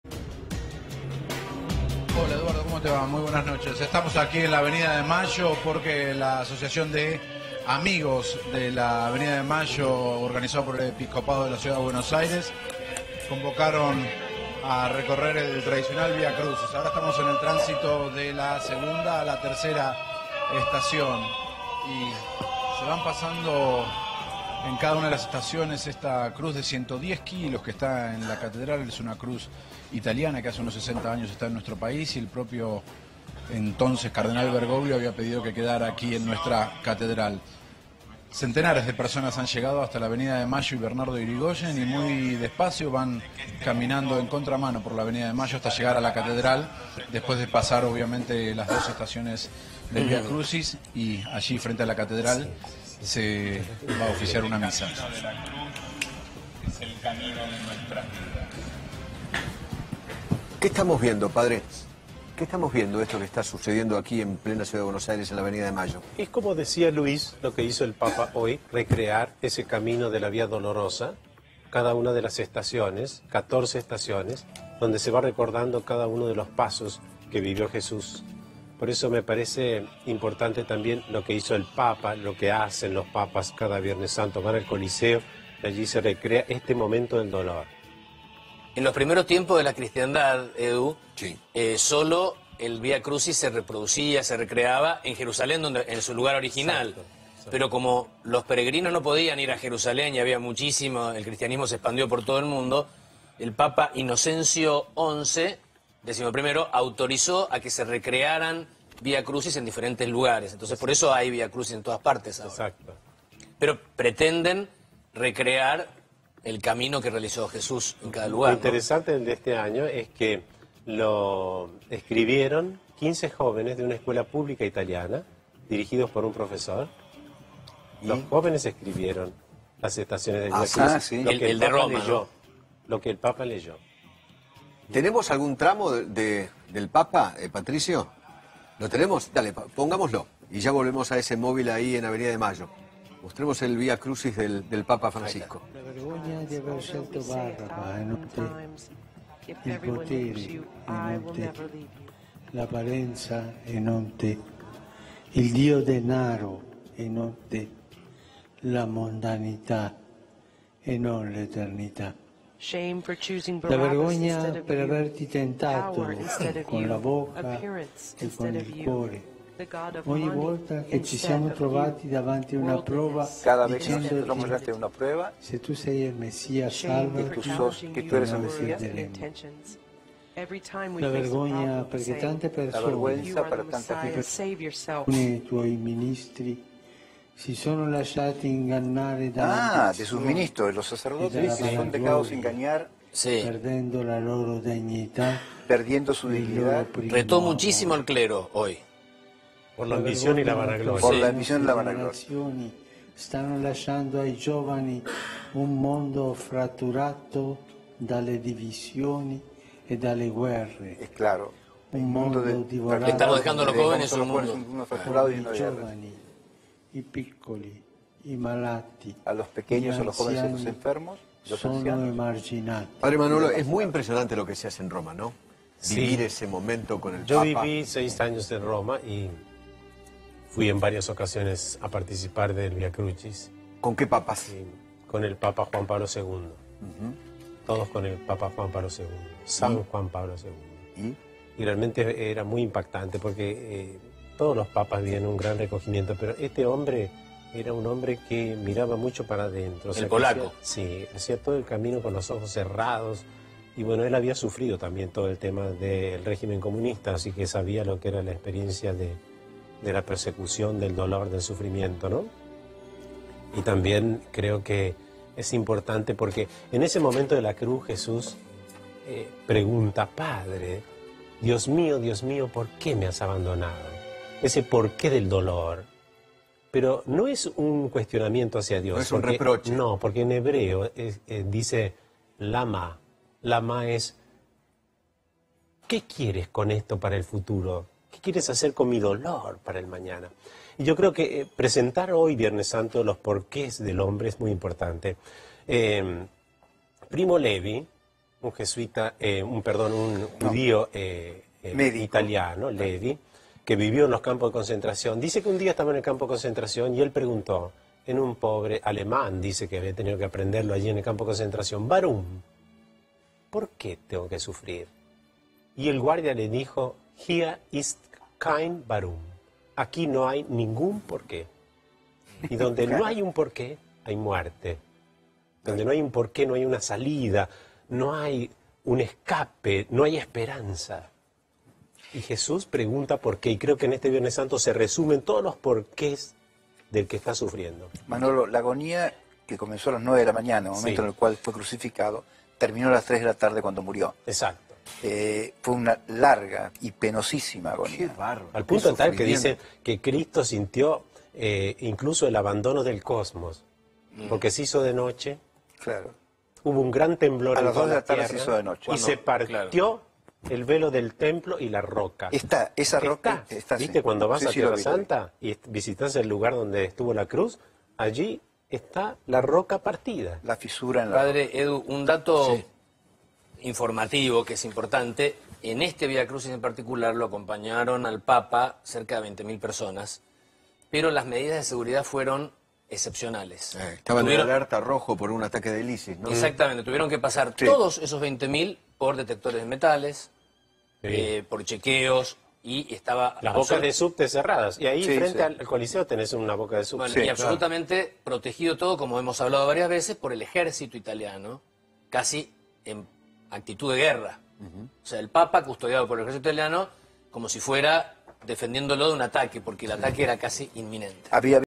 El arzobispo Mario Poli presidió el Vía Crucis por Avenida de Mayo
El arzobispo de la Ciudad de Buenos Aires, Mario Poli, presidió el Vía Crucis que se extendió por Avenida de Mayo hasta la Catedral, en el ritual del Viernes Santo.